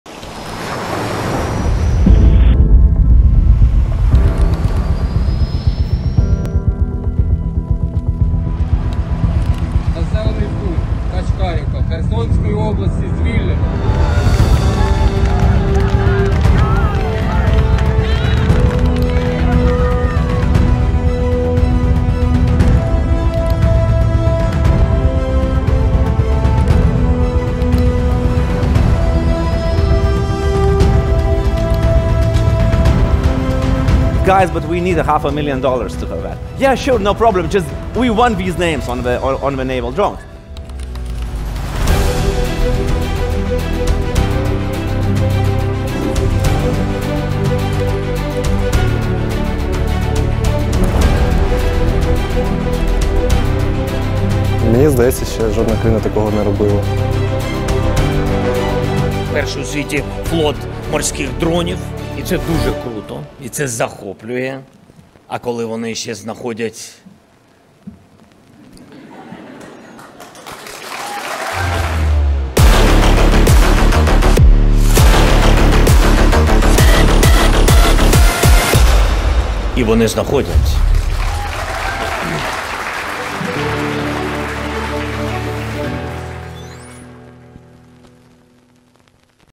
Флоту морських дронів — рік. Збір, який змінив хід подій у Чорному морі.
Флоту морських дронів — рік.

Збір, який змінив хід подій у Чорному морі. Який обʼєднав увесь світ. Україна, яка фактично не мала свого флоту, змусила окупантів забирати свої кораблі, ховати свої кораблі та відпускати свої кораблі… на дно🙂

Згадаймо, як це було.